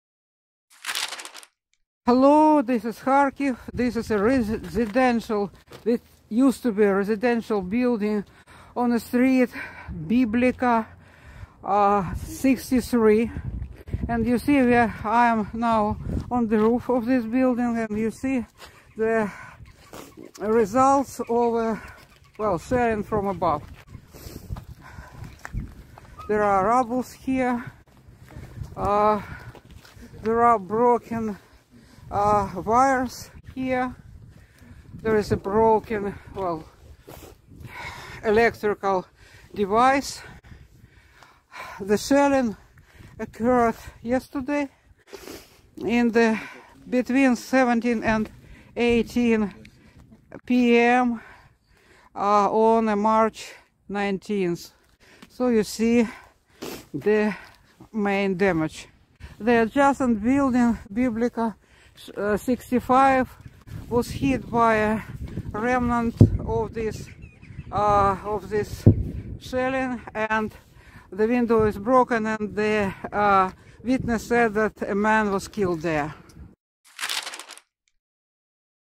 ウクライナ第2の都市ハリコフで撮影された、砲撃を受けた住宅の屋上（ナタリア・ズバルさん提供）
爆撃が続くウクライナ第2の都市ハリコフで、市民団体が戦争犯罪の証拠を残そうと、破壊された街並みを撮影しています。こちらの動画では穴が開いた屋根や切れた電線が散らばっているだけでなく、残骸が向かいの住宅に飛んで1が亡くなったということです。

詳しくは2022年4月7日付東京新聞朝刊で紹介しています。